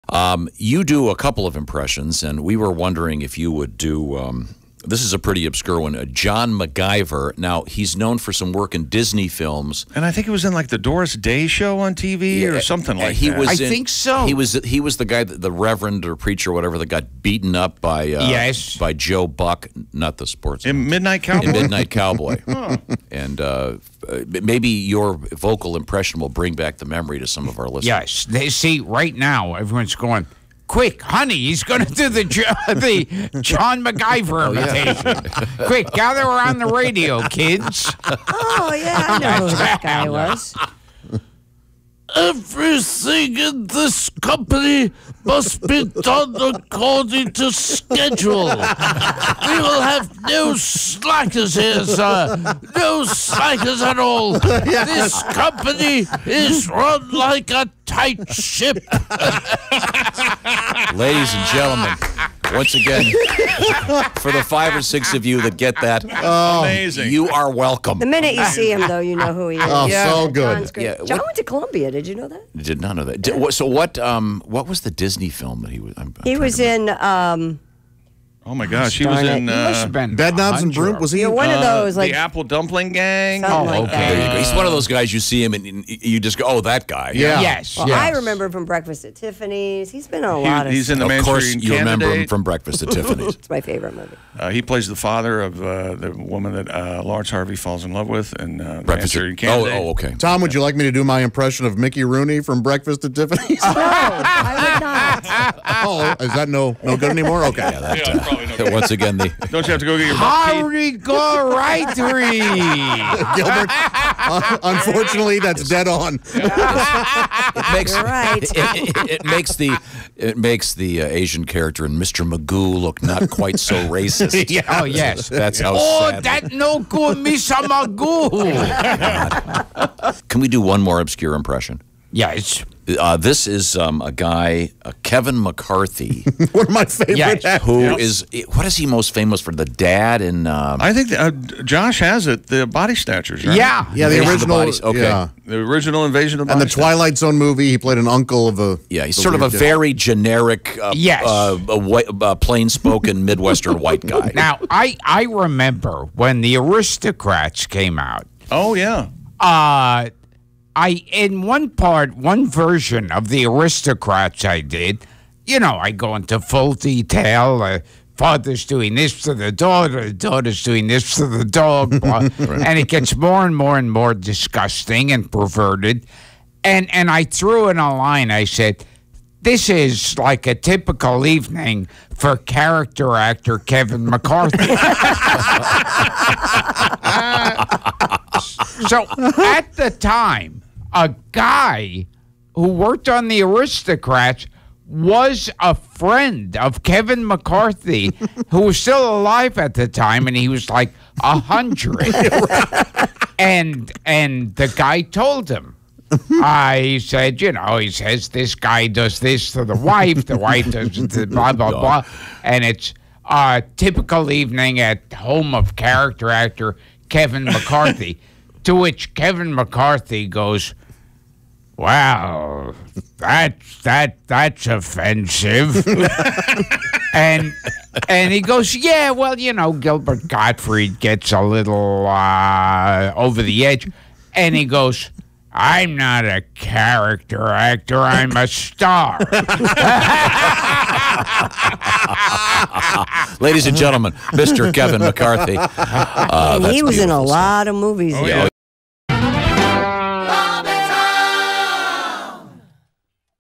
Comedian Gilbert Gottfried stops by with some obscure impressions.
Gilbert does his impressions of John McGiver and Kevin McCarthy